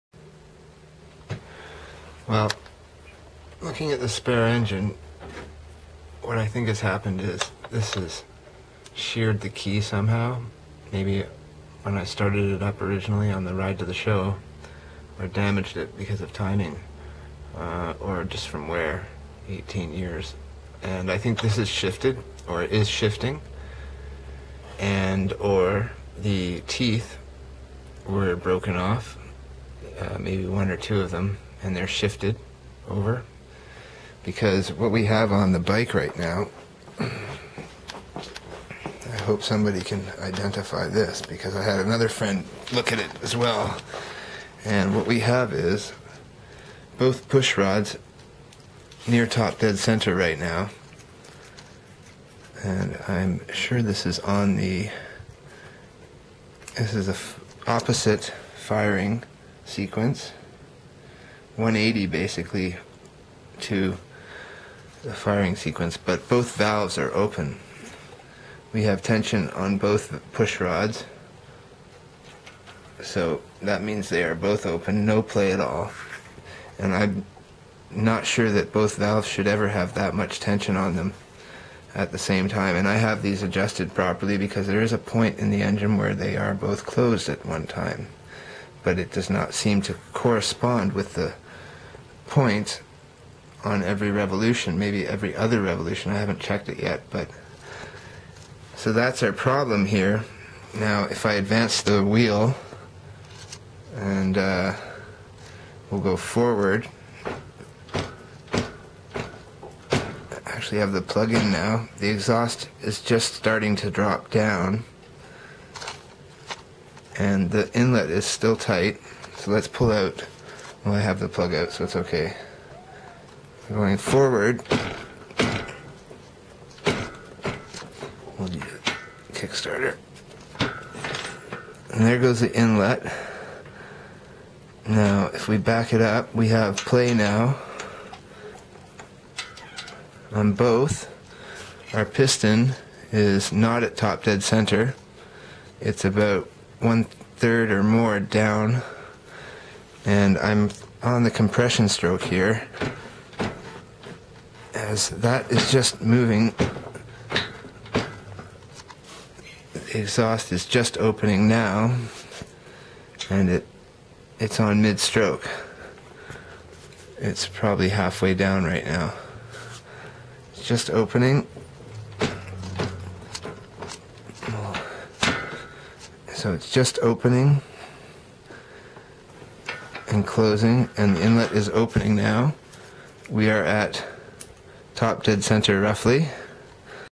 Valve timing error on BSA 441cafe, timing gear and crank key check next?
Had a pal over today and it is confirmed either timed or every other stroke the exhaust and inlet are remaining open at tdc on intake. I can find tdc and there is play on tappets then after a few kicks I can also find the valves both with pushrod pressure at what looks to be 180 of the points setting thus the blowing out of carb during a stage of kick.
-Possible causes I can guess: Timing gear teeth have broken off and are in situ shifted over.
-the key on crankshaft has sheared or is shearing and the gear is slipping or rotating. I can back up piston to tdc with slack possible audio click from timing area in case.